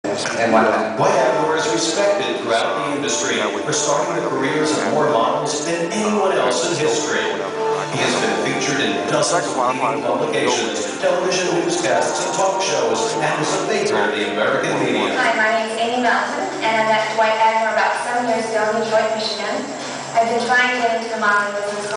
Inside Look At The Modeling Industry!
Here you will travel with Lawrence O'neal as he takes an inside look into the modeling industry & seeks to help you get your foot into the door, while following your dreams!